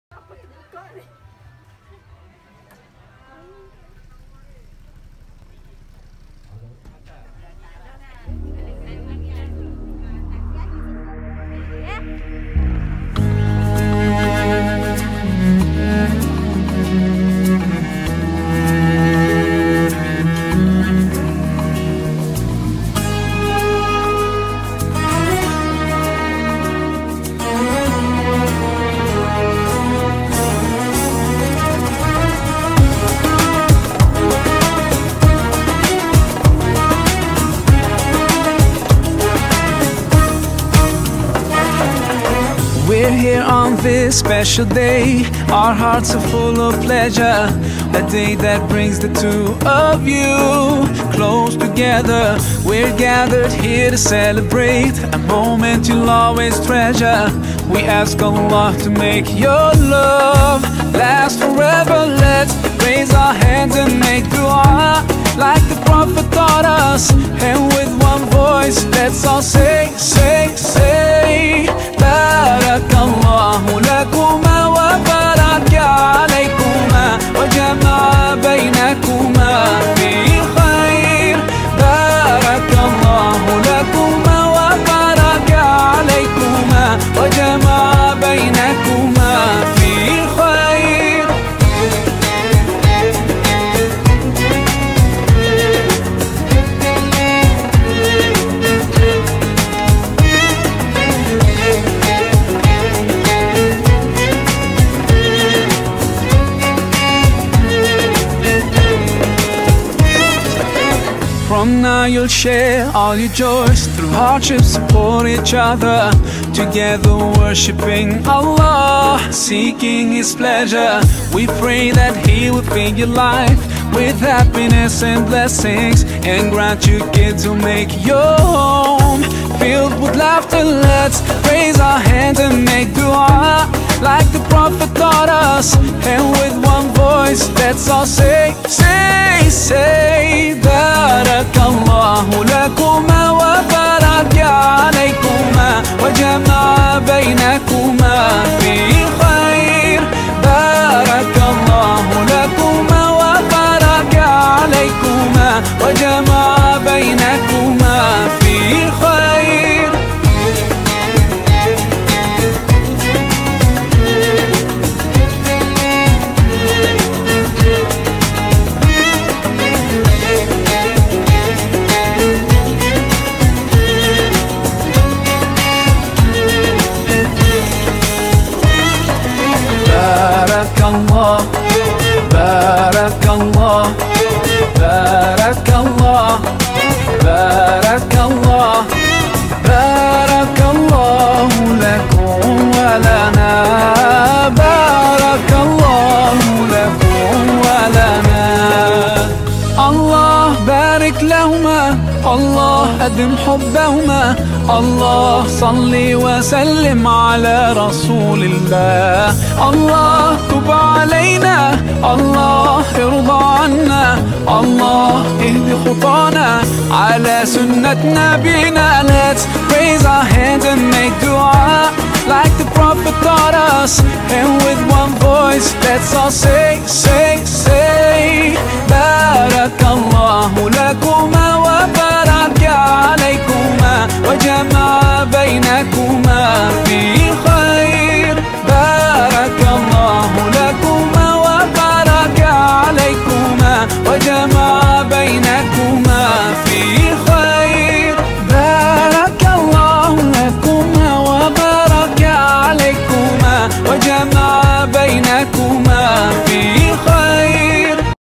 Raikan Cinta Shafiq & Amelia 01.02.2022
Bertandang ke rumah pengantin lelaki di Tanjung Karang.